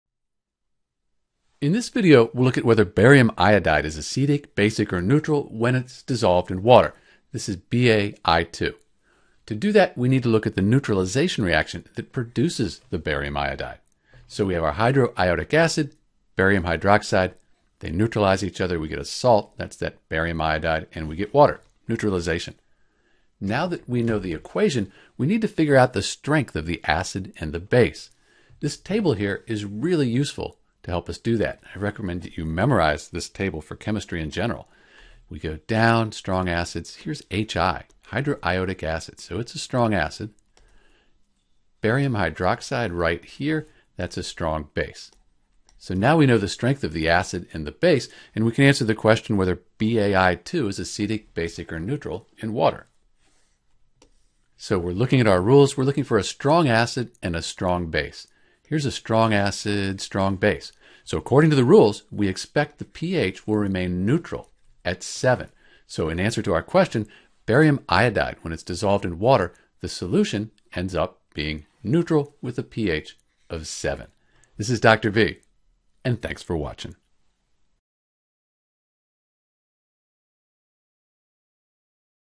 Is BaI2 acidic, basic, or neutral (dissolved in water)?
To tell if BaI2 (Barium iodide) forms an acidic, basic (alkaline), or neutral solution we can use these three simple rules along with the neutralization reaction that formed BaI2 .

First we need to figure out the acid and base that were neutralized to form Barium iodide. The equation for BaI2 is:

HI + Ba(OH)2 = BaI2 + H2O
It is also useful to have memorized the common strong acids and bases to determine whether BaI2 acts as an acid or base in water (or if it forms a neutral solution).

Strong acids: HCl, H2SO4, HNO3, HBr, HI, HClO4
Weak acids: HF, CH3COOH, H2CO3, H3PO4, HNO2, H2SO3
Strong Bases: LiOH, NaOH, KOH, Ca(OH)2, Sr(OH)2, Ba(OH)2
Weak Bases: NH3, NH4OH
Note that we are talking about whether BaI2 is an acid, base, or neutral when dissolved in water.

Based on these rules, the solution of BaI2 dissolved in water is neutral.